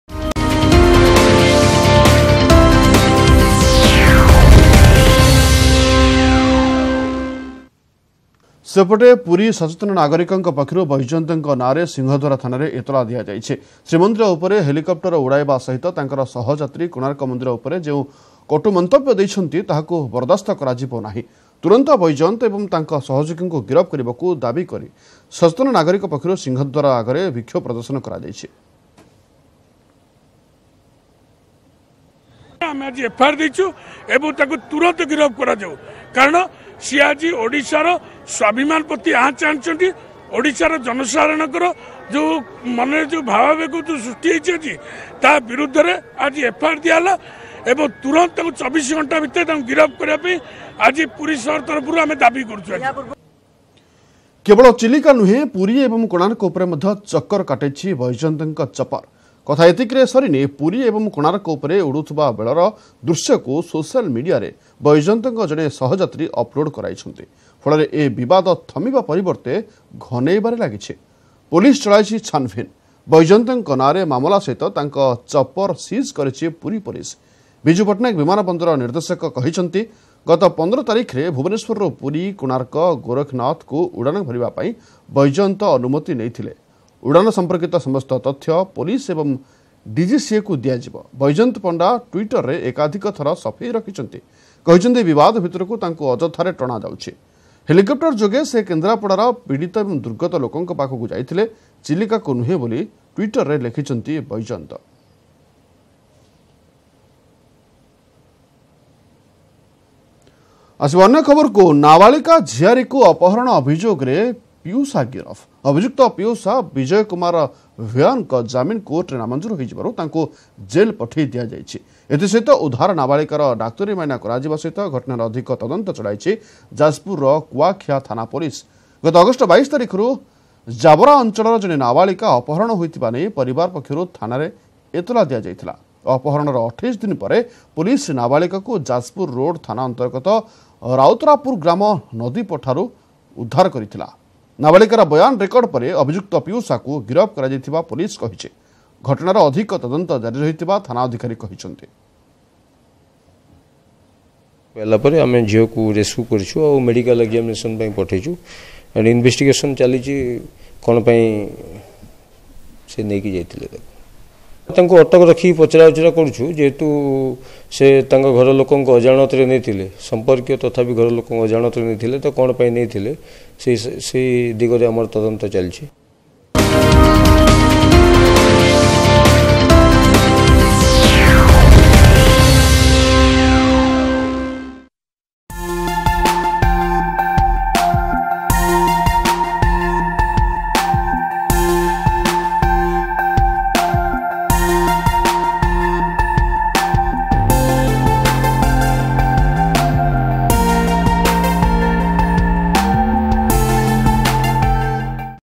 ସଚେତନ ନାଗରିକ ପକ୍ଷରୁ ଥାନାରେ ଏତଲା ,ବୈଜୟନ୍ତ ଏବଂ ସହଯୋଗୀଙ୍କୁ ଗିରଫ ଦାବି | AAMARI ODISHA
ଶ୍ରୀମନ୍ଦିର ଏବଂ କୋଣାର୍କ ମନ୍ଦିର ଉପରେ ହେଲିକପଟର ଉଡ଼ାଇବାକୁ ନେଇ ସଚେତନ ନାଗରିକ ପକ୍ଷରୁ ସିଂହଦ୍ୱାର ଥାନାରେ ଏତଲା ,ବୈଜୟନ୍ତ ଏବଂ ସହଯୋଗୀଙ୍କୁ ଗିରଫ କରିବାକୁ ଦାବି.